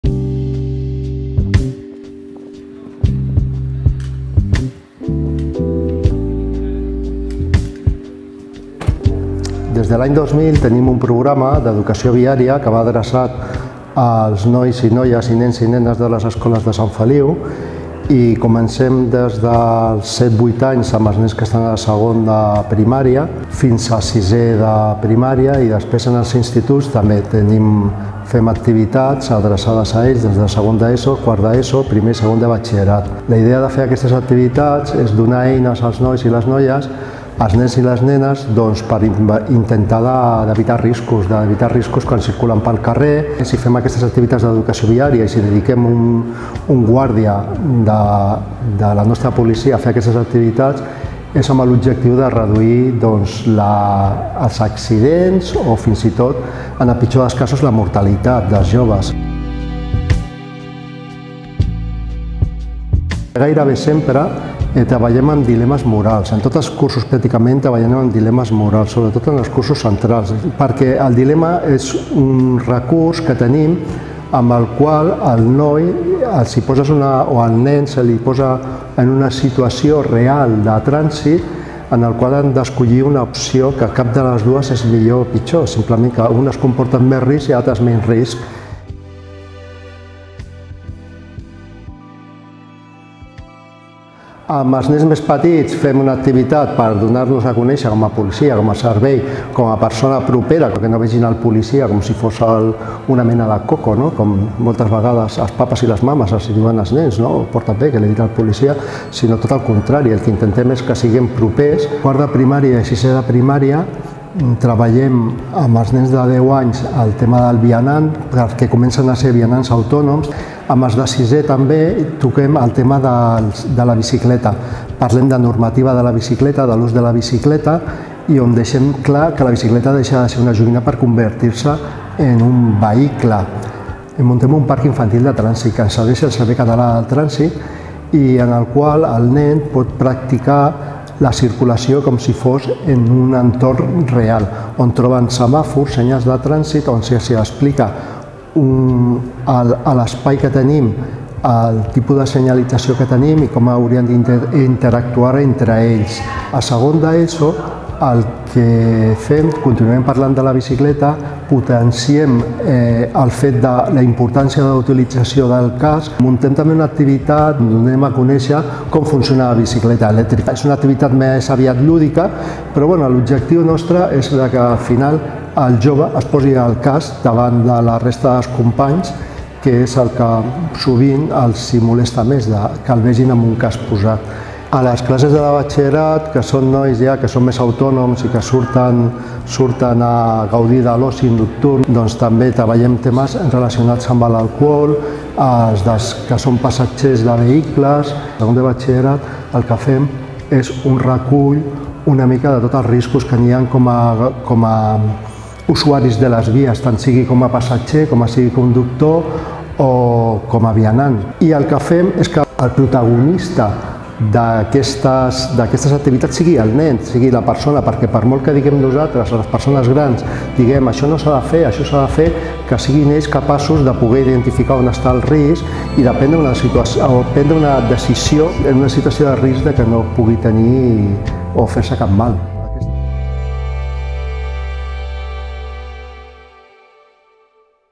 Educació viària amb Policia Local de Sant Feliu
La Policia Local de SantFeliu impulsa des de l'any 2000 un programa d'educació viària amb activitats formatives sobre seguretat vial als centres escolars i el parc infantil de trànsit.

L'Ajuntament destina un agent per ensenyar no només coneixements i normes de seguretat viària, sinó sobretot actituds preventives davant situacions reals en l'entorn més pròxim.